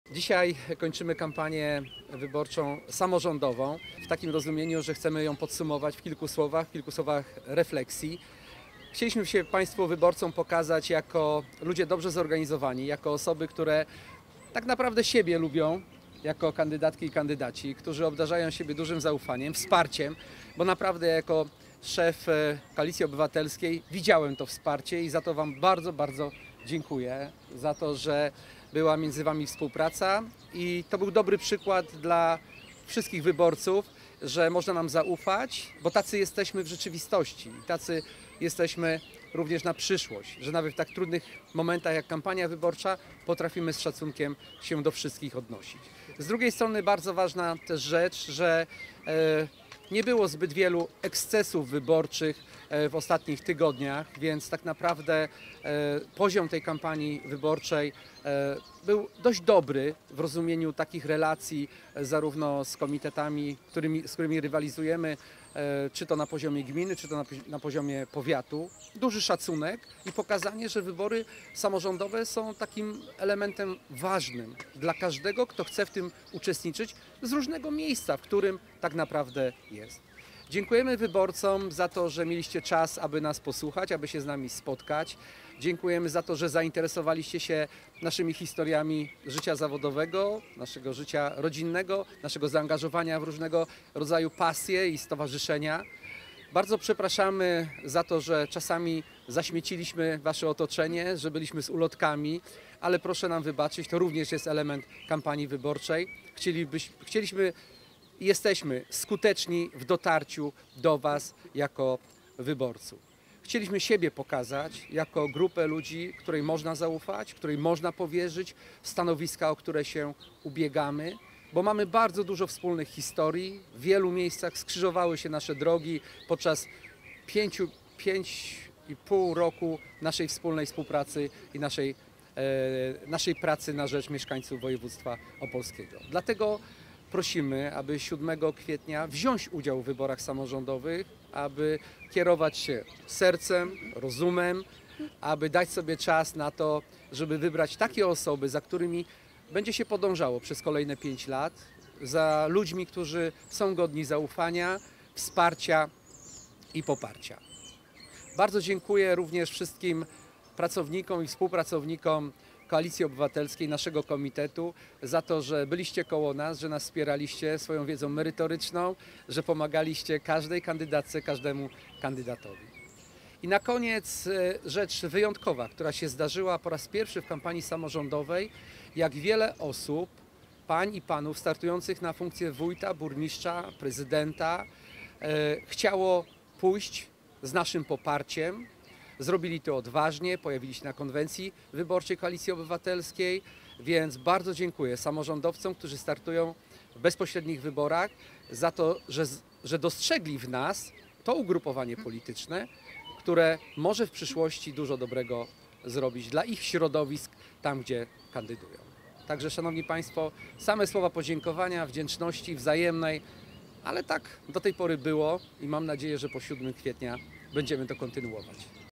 Kampania KO na finiszu
Przedstawiciele opolskiej Koalicji Obywatelskiej podsumowali całą swoją  samorządową kampanię wyborczą. Była to okazja do przypomnienia o sztandarowych postulatach koalicji oraz podziękowania za współpracę. W konferencji prasowej uczestniczyli kandydaci do rady miasta oraz sejmiku województwa opolskiego.
Wybory samorządowe odbędą się już 7 kwietnia w niedzielę.